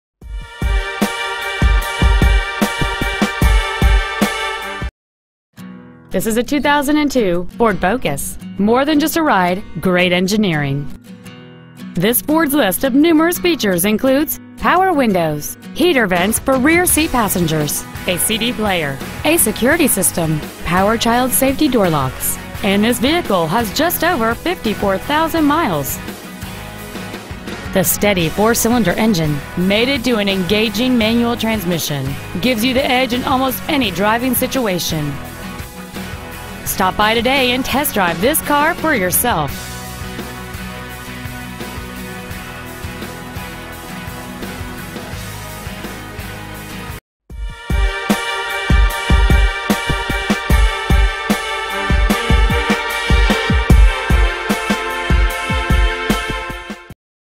2002 Ford Focus Miamisburg OH
We are proud to present this 2002 Ford Model: Focus

 Year: 2002
 Make: Ford
 Model: Focus
 Engine: 4 Cyl - 2.0L
 Trans.: Manual
 Exterior: Pitch Black
 Miles: 54,672
 Interior: Dark Charcoal w/Cloth Low Back Bucket Seats

 125 Alexandersville Rd.

 Used 2002 Ford Focus Miamisburg OH